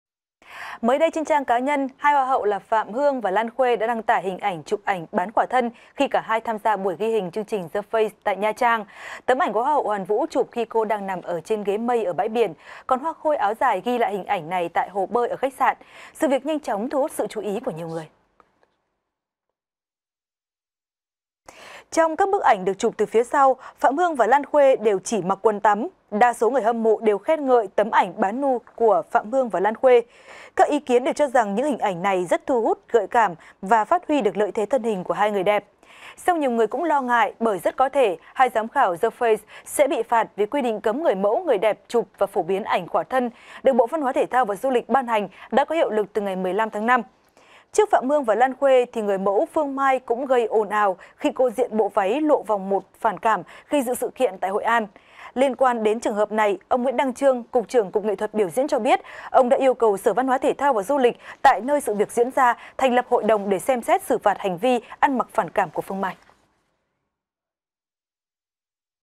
(VTC14)_Phạm Hương, Lan Khuê có bị phạt vì khoe ảnh bán khỏa thân?
(VTC14) - Trên trang cá nhân, hai hoa hậu Phạm Hương và Lan Khuê đã đăng tải hình ảnh chụp bán khỏa thân khi cả hai tham gia buổi ghi hình chương trình The Face tại Nha Trang. Nhiều người lo ngại, hai giám khảoThe Face sẽ bị phạt vì quy định cấm người mẫu, người đẹp chụp và phổ biến ảnh khỏa thân được Bộ Văn hóa, Thể thao và Du lịch ban hành đã có hiệu lực từ ngày 15/5.